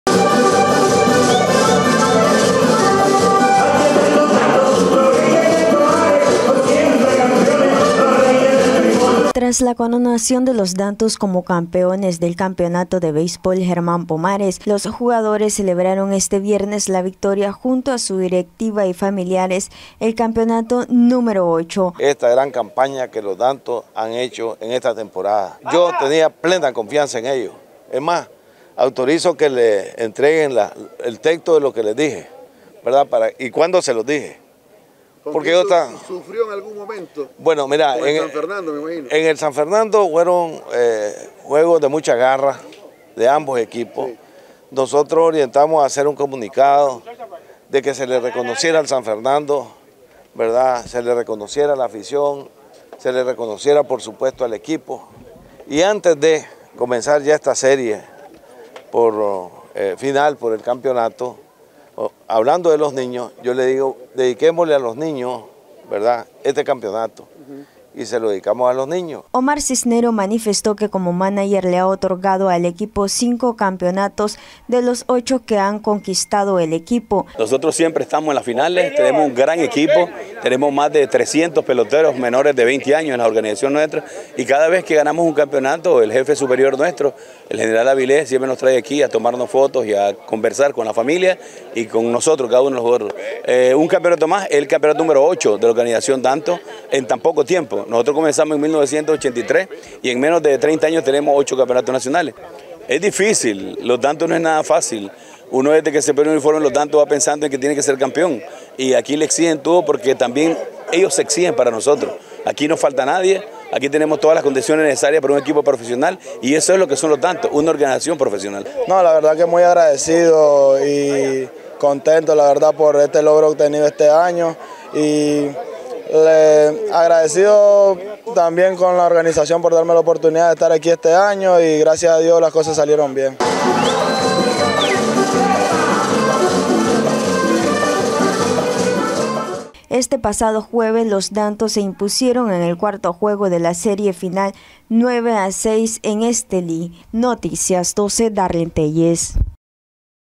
Noticias12Nicaragua Equipo de los Dantos continúa celebrando su campeonato número ocho
Equipo de los Dantos continúa celebrando su campeonato número ocho